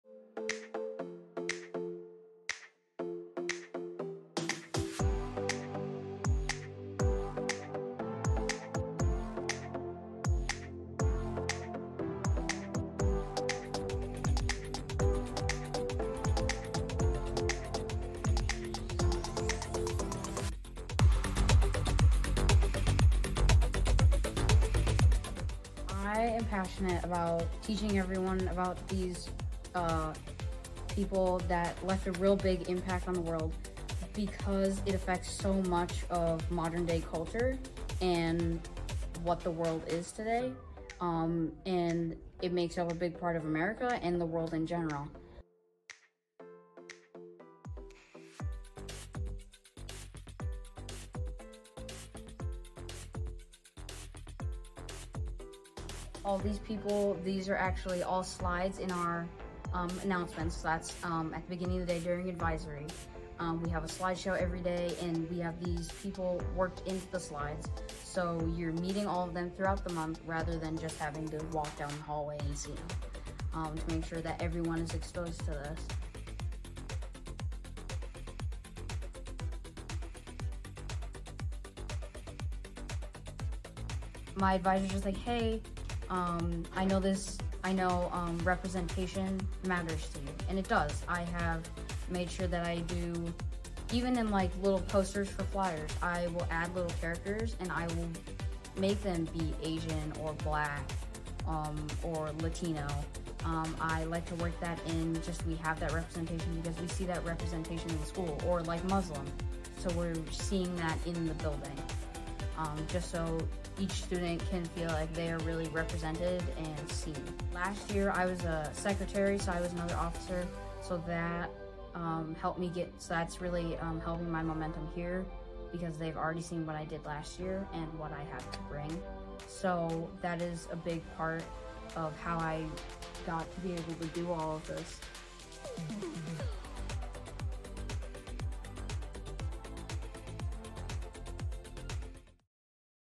Black History Month 2022 Project Spotlight
Throughout the month of February, students across the Shakopee Public School district have expanded their awareness of black history month, while engaging in thoughtful and meaningful activities, many of which have been led by their peers.